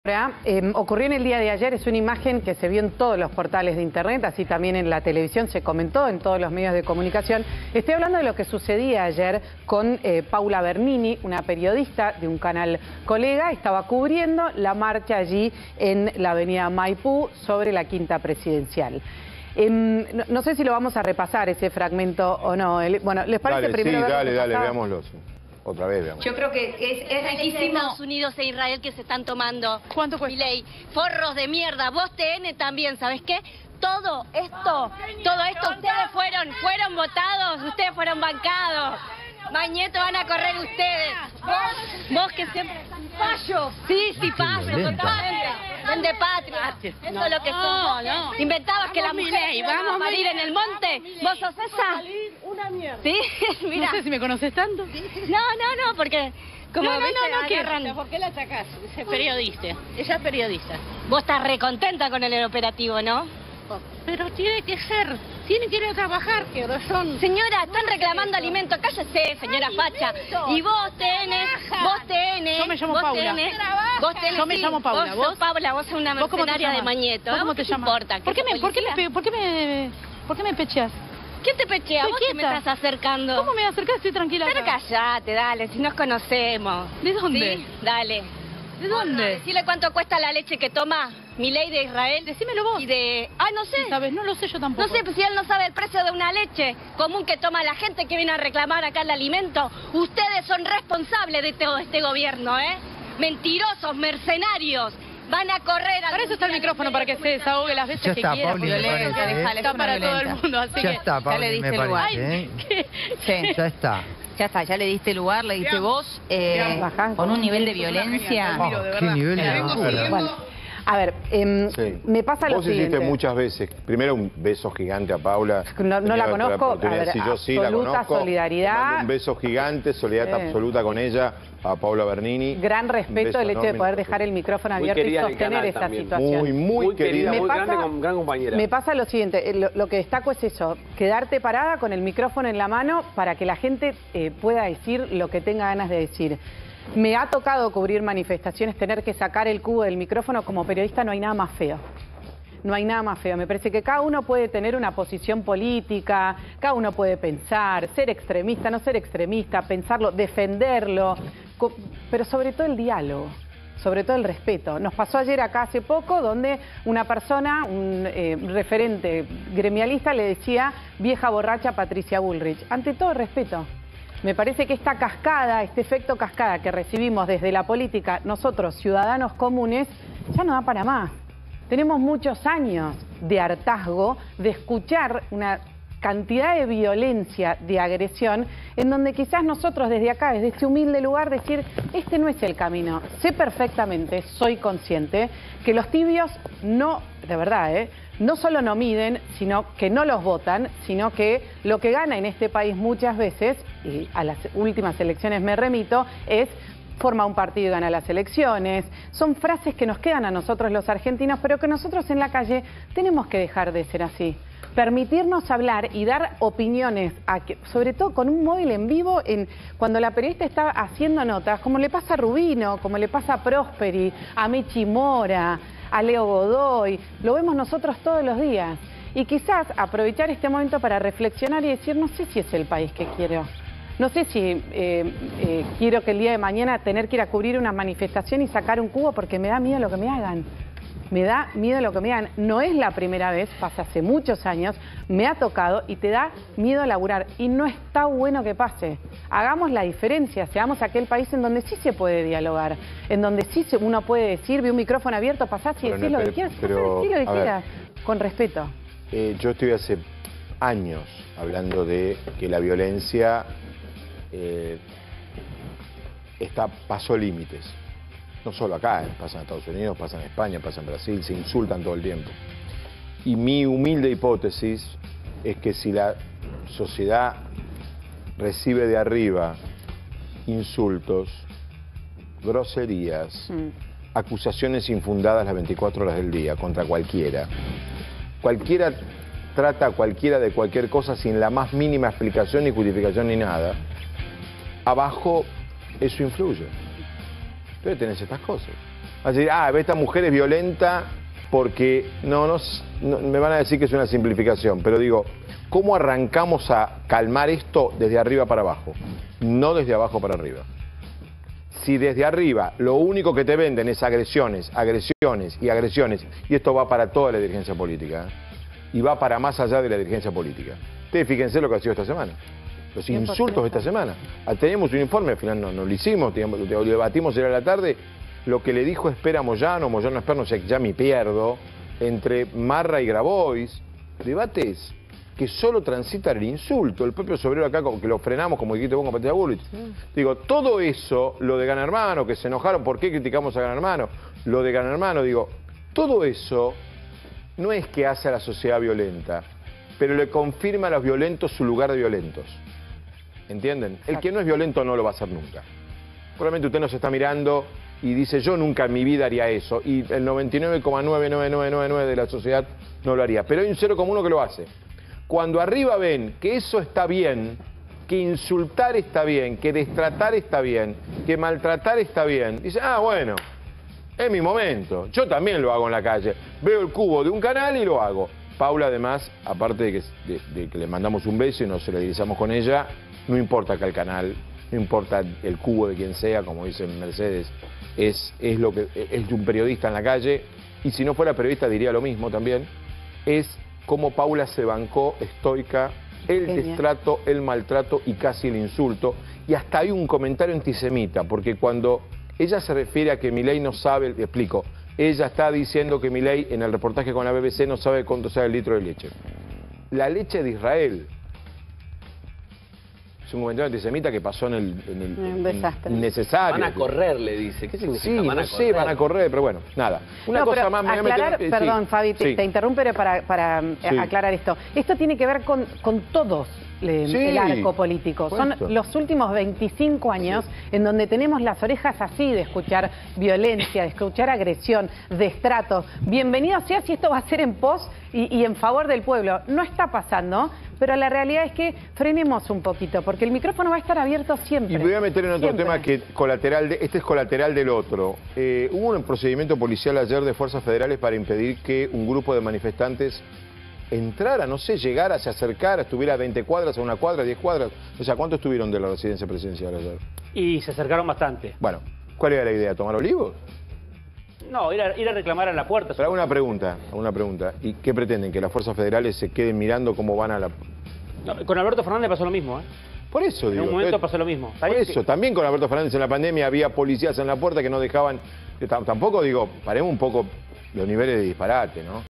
🔴 La terrible agresión que sufrió Paula Bernini durante un móvil en la calle
👉 Una mujer se aproximó a la periodista de TN mientras estaba realizando una salida en vivo y la increpó de manera violenta.